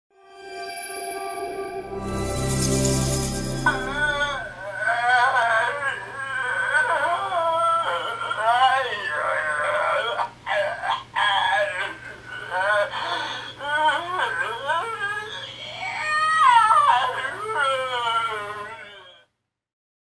Halloween Haunters | Crawling Human Zombie
Halloween Haunters
Animated 45" Crawling Zombie Torso